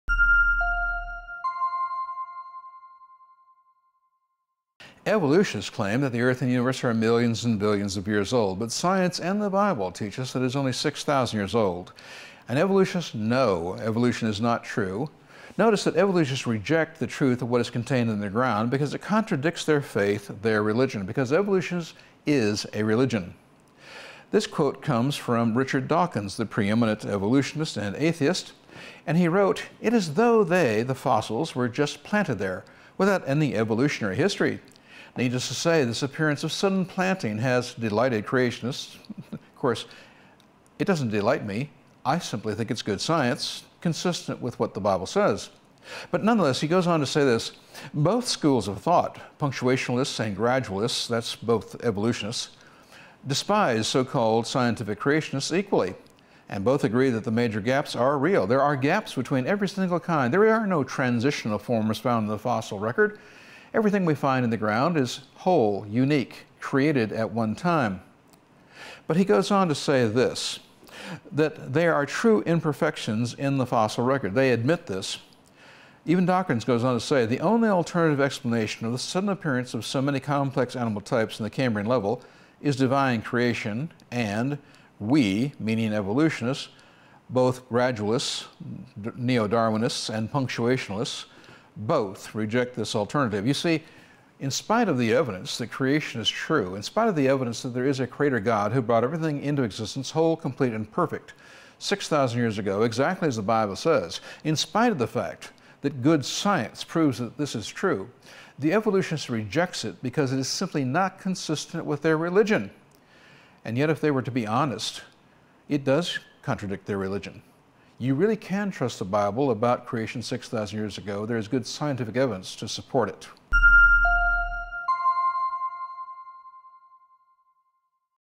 Sudden Appearance of Fossils
“It is as though they [fossils] were just planted there, without any evolutionary history. Needless to say this appearance of sudden planting has delighted creationists. ... Both schools of thought ['Punctuationists' and 'Gradualists'] despise so-called scientific creationists equally, and both agree that the major gaps are real, that they are true imperfections in the fossil record. The only alternative explanation of the sudden appearance of so many complex animal types in the Cambrian era is divine creation and both [schools] would reject this alternative.” Richard Dawkins, The Blind Watchmaker, W. W. Norton & Company, New York, 1996, p. 229-230)